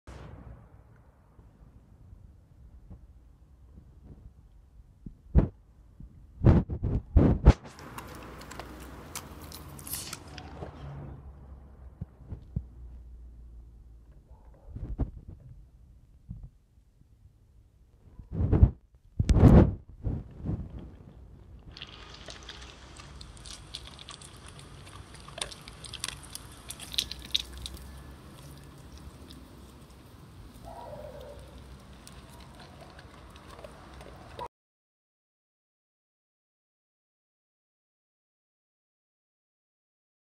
Dogs Eating Food
Golden Retriever and Dachshund eating their puppy chow,
Lol just kidding please do both! Discord link at end of this video! Thanks 4 watching!